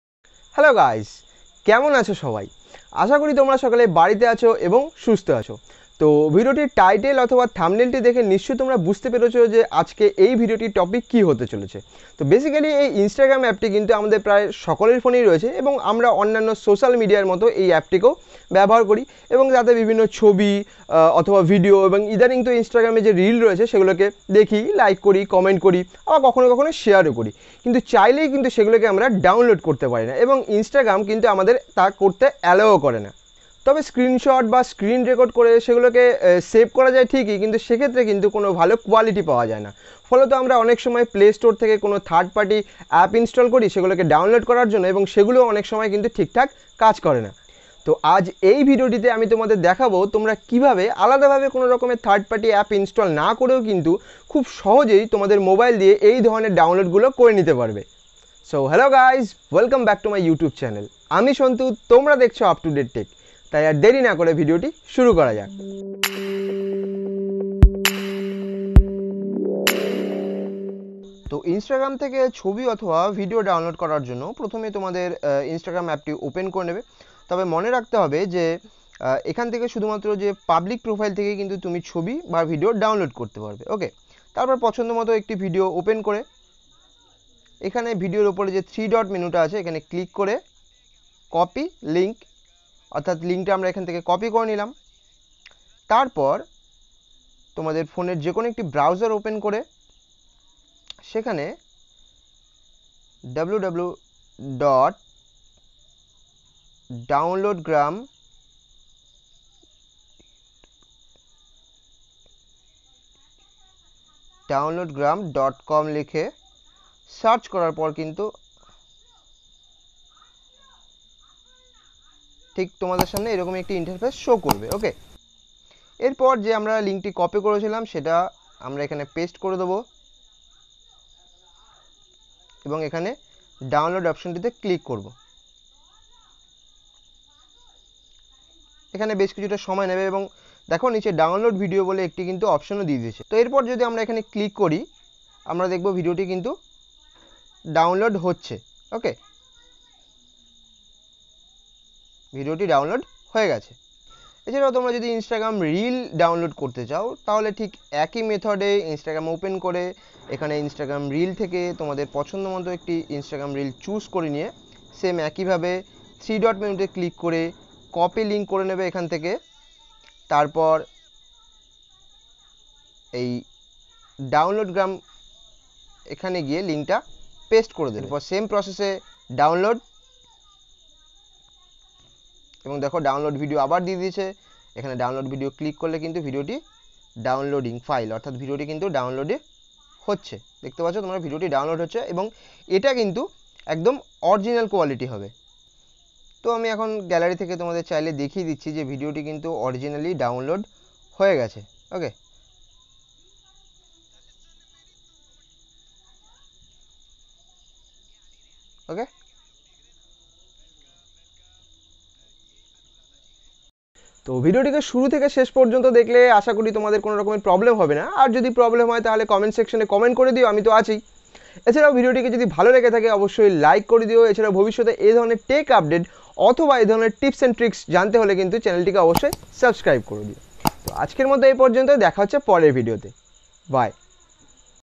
How to download Instagram photos, videos, Instagram reels on Android without any app || Update tech
As others social platforms Instagram also has so many videos,photos and now a days Instagram reels are also very popular.we often watch this kind of photos and videos but we can't download that from Instagram because Instagram don't allow us to do that  but in this video I'll show you How to download Instagram photos/videos/reels on Android without any app . You can also learn about

How to save Instagram stories
How to save videos from Instagram
how to download Instagram reels
save Instagram videos in Bangla

So guys please watch this content till the end download your favourite Instagram videos or photos easily .